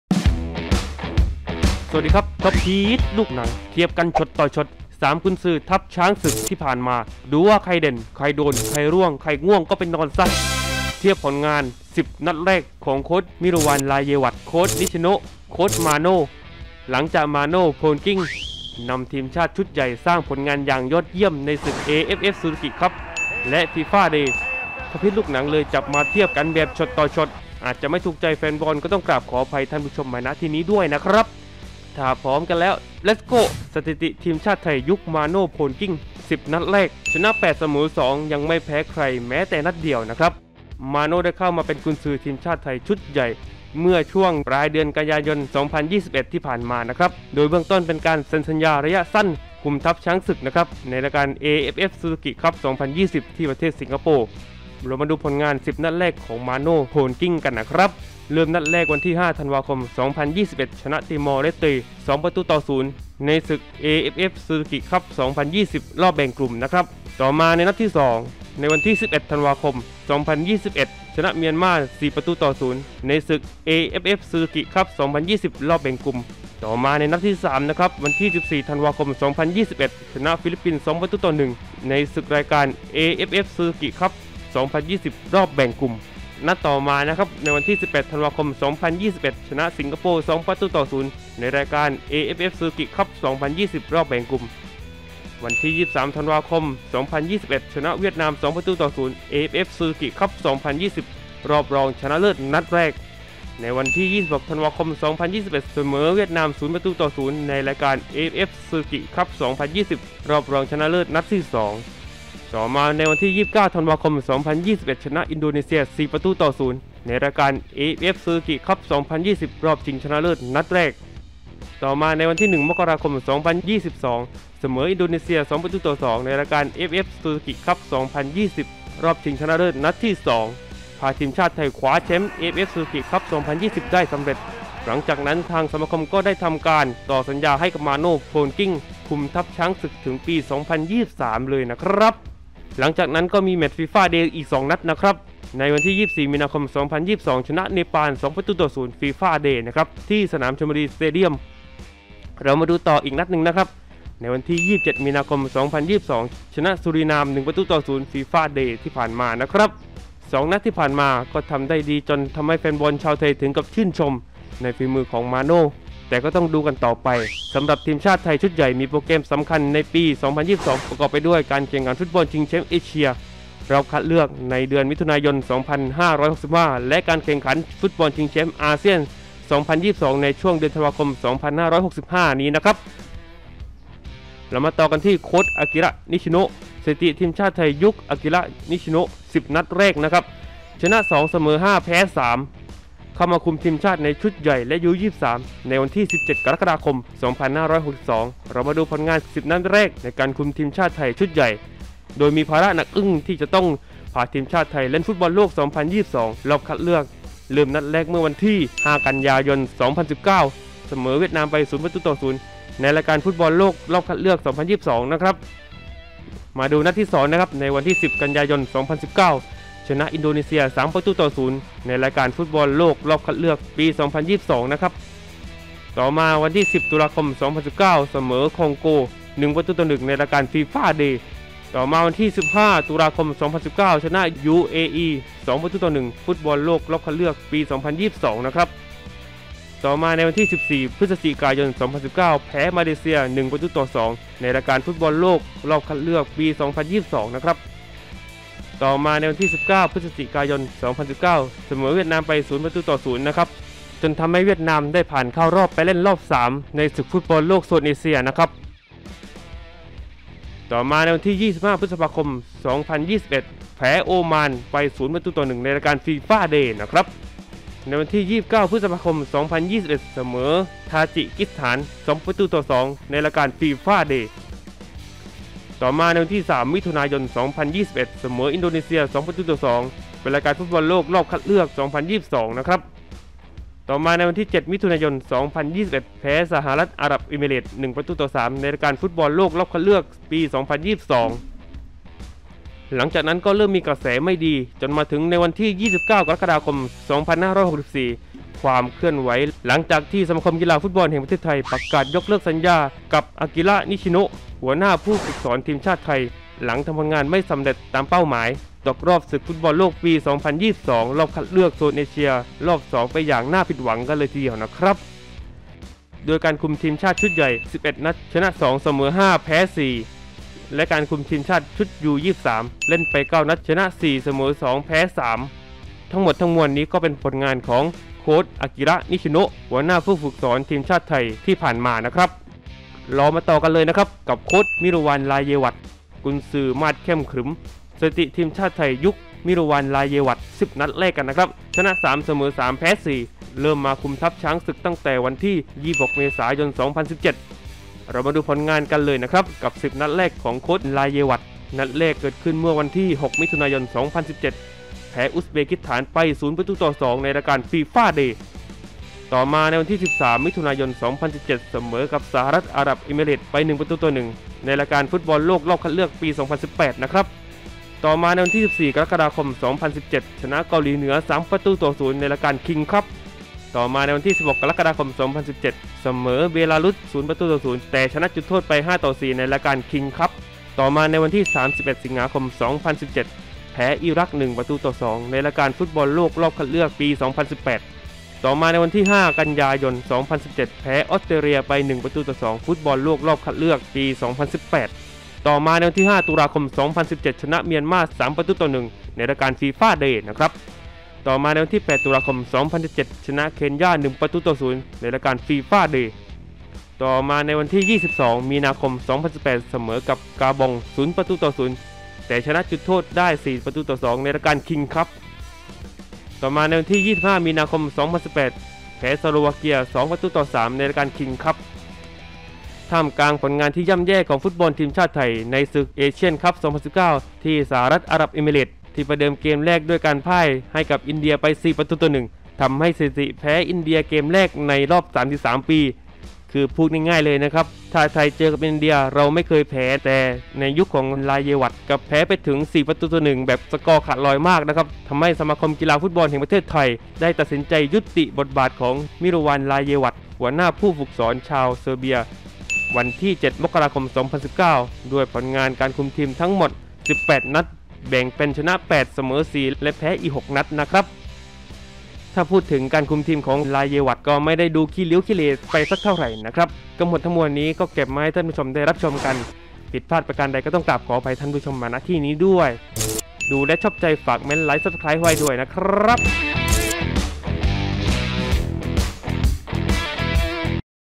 เทียบกันช็อตต่อช็อต 3 เฮดโค้ชทีมชาติไทย/ยุคมาโน่/ยุคนิชิโนะ/ยุคราเยวัช ใครถูกใจแฟนช้างศึกมากที่สุด
สวัสดีคับ ท็อปฮิตลูกหนัง เทียบกันช็อตต่อช็อต 3 กุนซือทัพช้างศึกที่ผ่านมา ดูว่าใครเด่น ใครโดน ใครร่วง ใครง่วงก็ไปนอนซะๆ พร่ามๆ เทียบผลงาน 10นัดแรก ของโค้ช ราเยวัช โค้ช นิชิโนะ โค้ชมาโน่ ผิดพลาดประการณ์ใด ก็ต้องกราบขออภัยท่านผู้ชมด้วยนะคับ
1ซับ 1LIKE = คุณได้สนับสนุนผมแล้วครับ

เพจท็อปฮิตลูกหนัง ไว้คุยกับผมได้ นะจ๊ะ [ขอมาหลวงจัดได้]